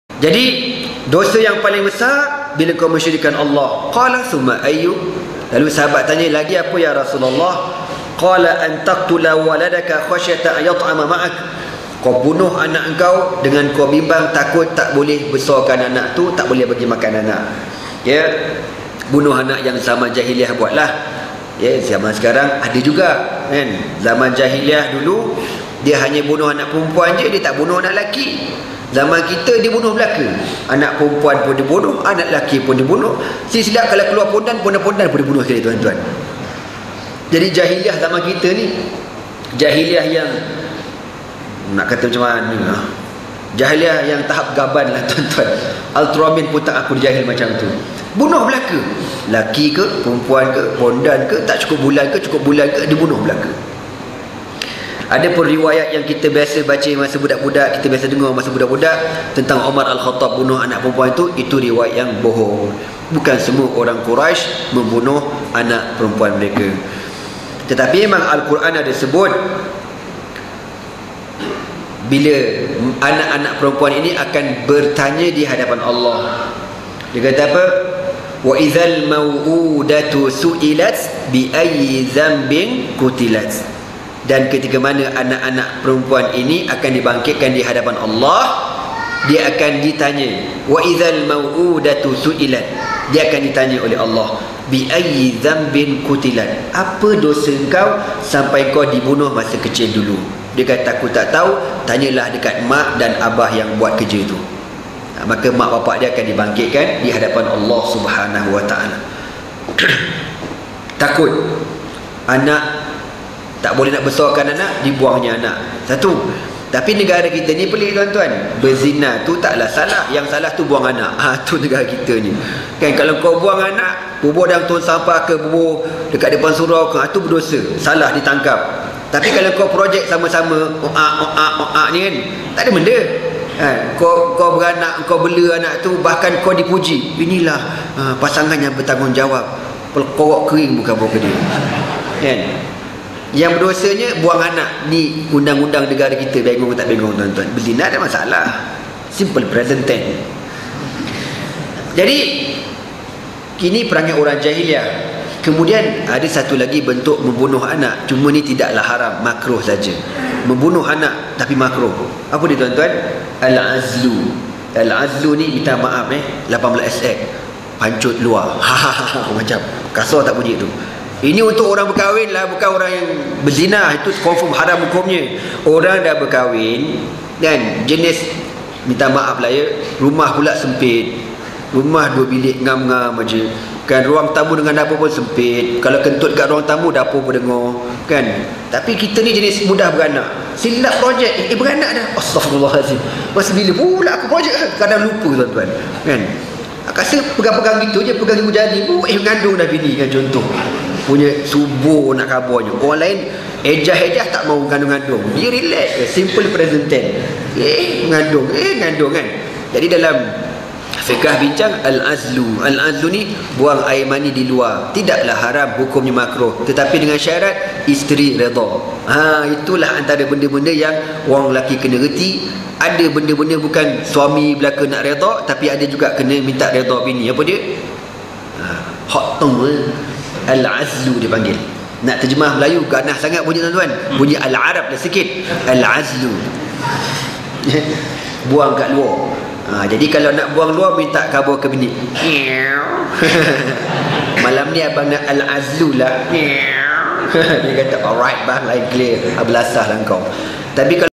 Ustaz Nazmi Karim: Bunuh anak .....yg makruh :))
Ustaz Muhammad Nazmi Karim |
Tafsir Al-Quran (Surah Al-Furqan Ayat 63-77)-Bhg 3 |
Surau Ar Rahmah |
Seksyen 8, Shah Alam |
14 Sya'ban 1433H